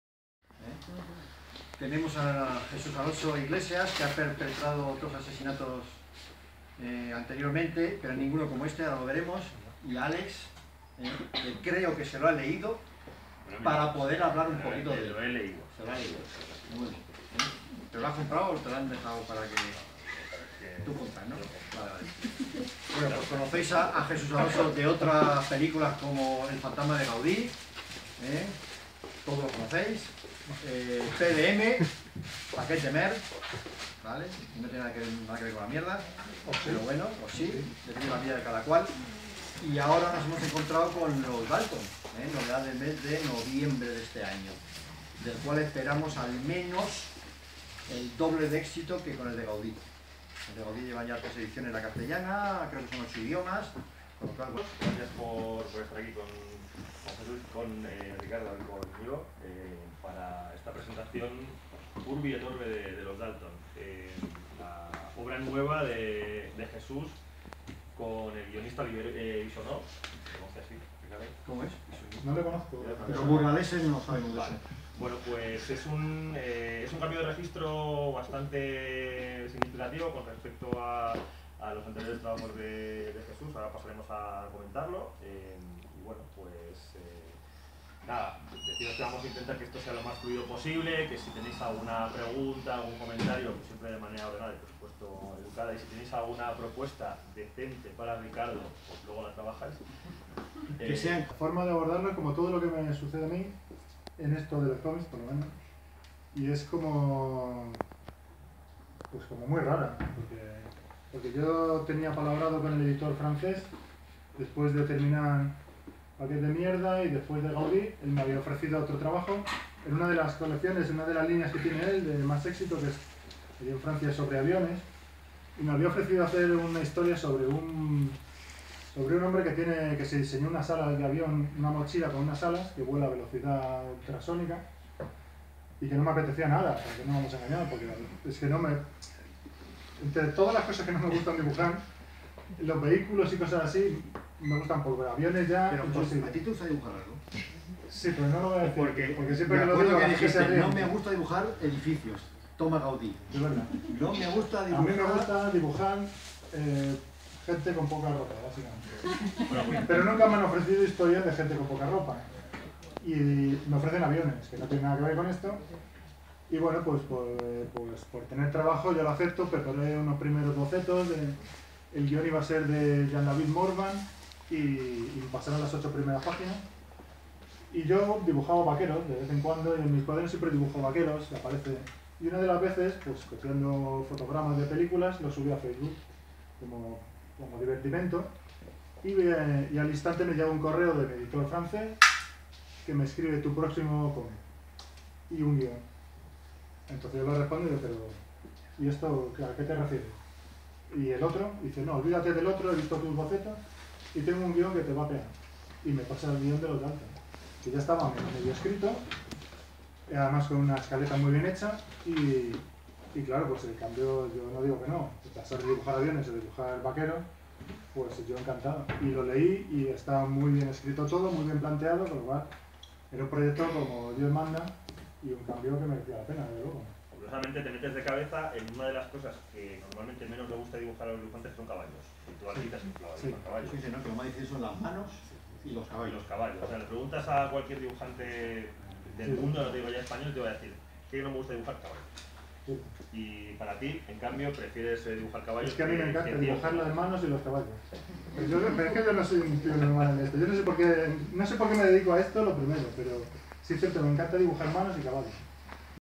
Presentación Cómic "Los Dalton". Jesús Alonso Iglesias. En Omega Center
Presentación del Cómic "Los Dalton" en Omega Center, en la calle Estrella de Madrid